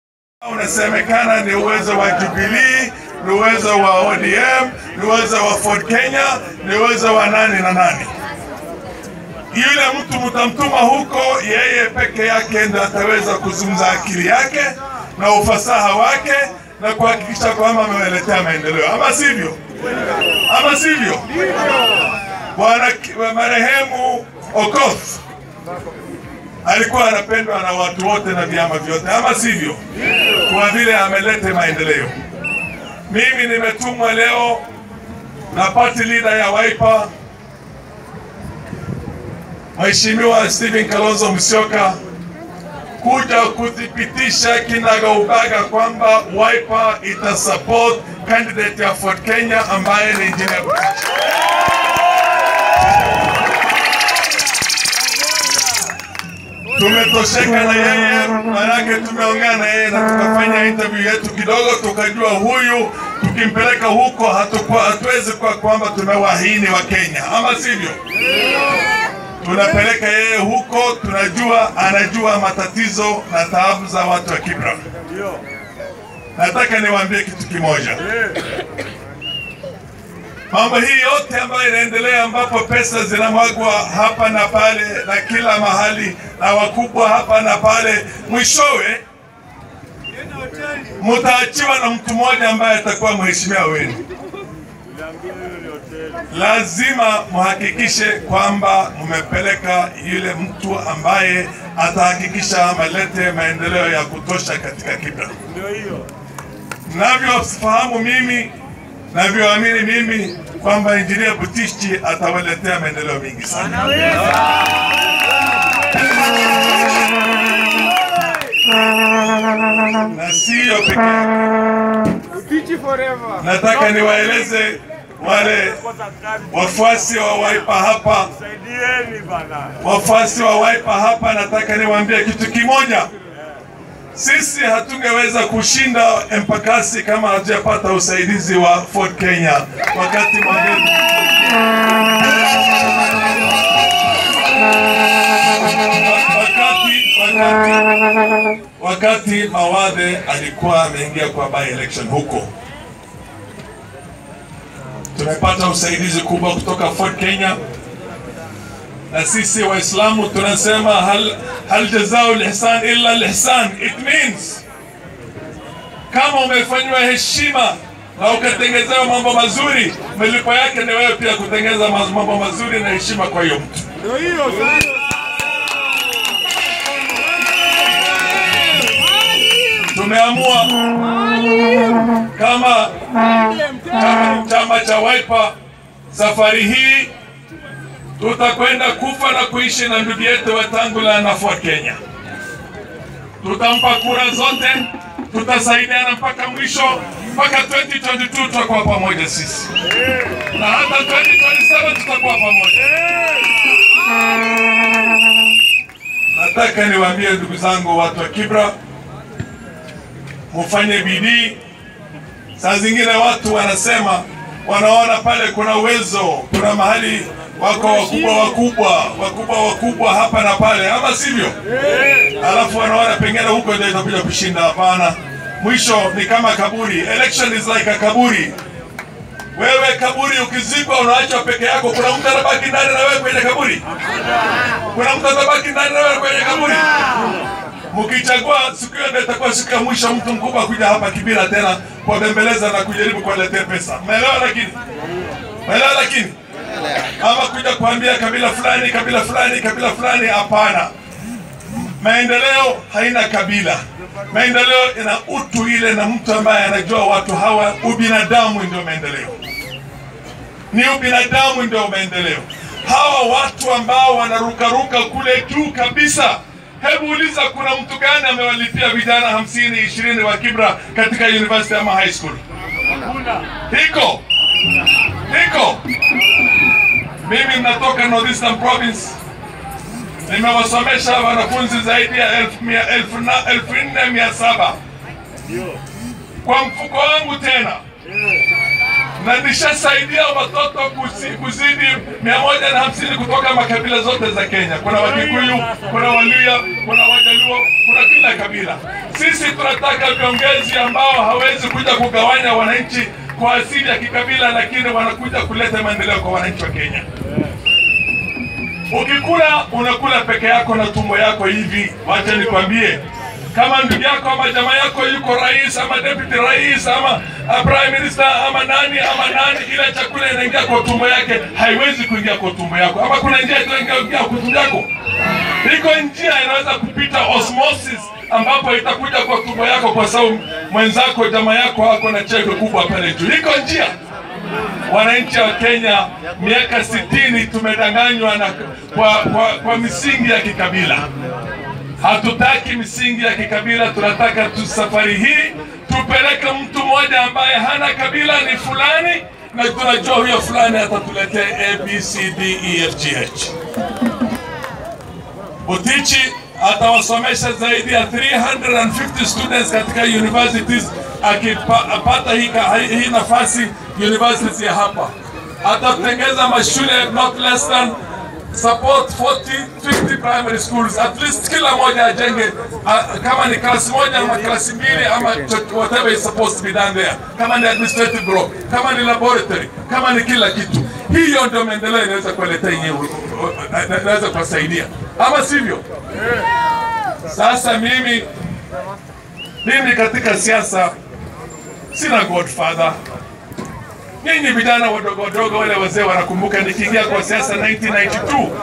6 October 2019
Wiper Party officially endorse Ford Kenya Kibra candidate